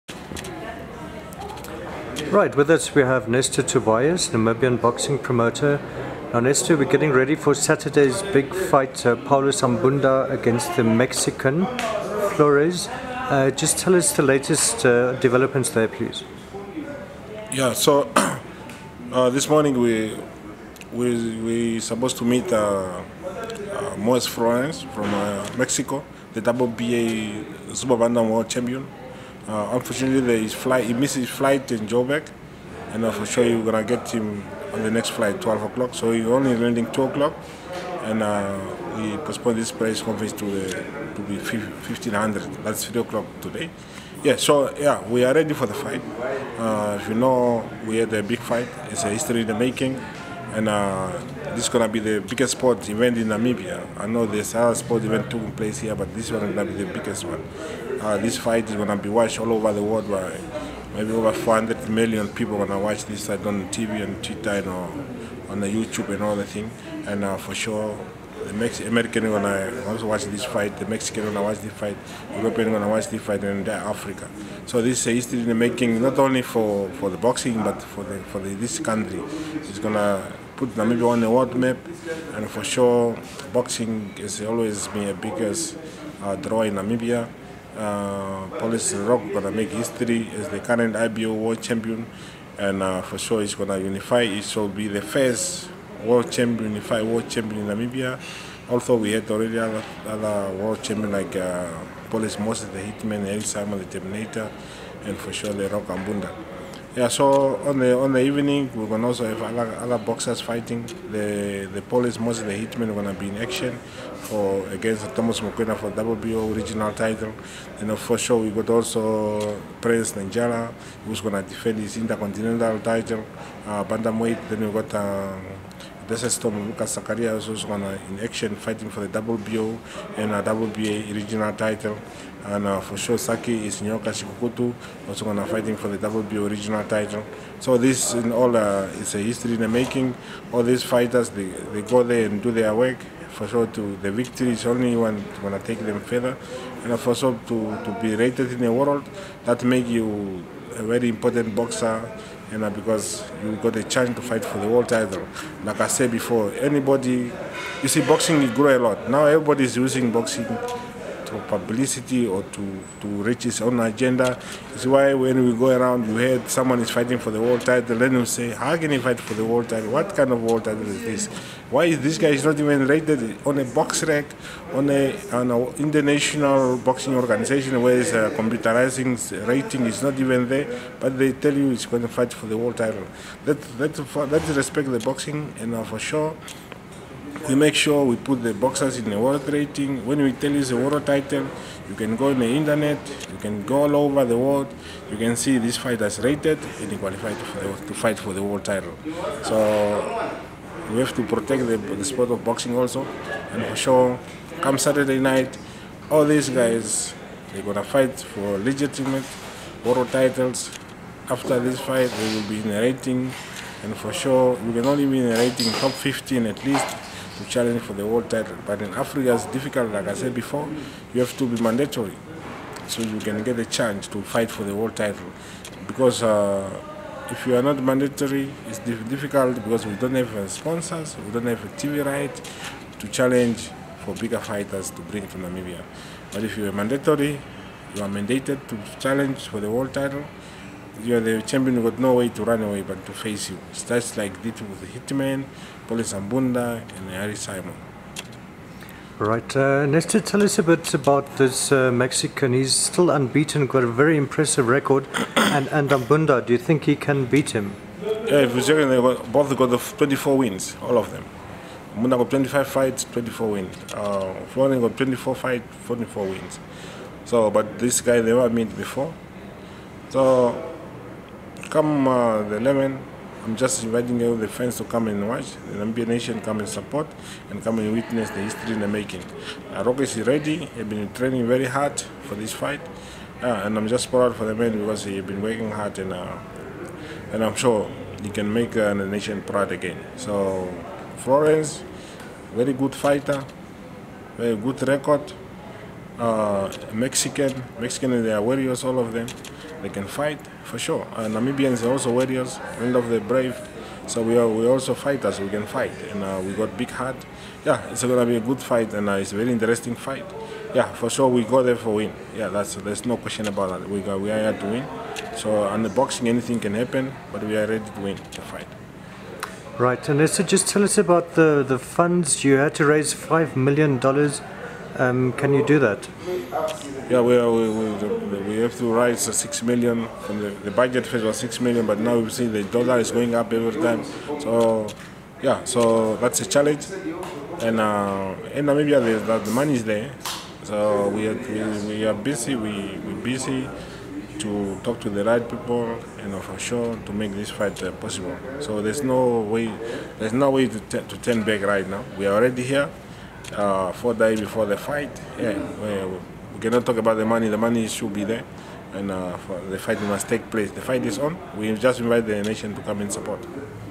Nestor Tobias
Namibian boxing promoter Nestor Tobias talks about the Super Fight between Moises Flores and Paulus Ambunda.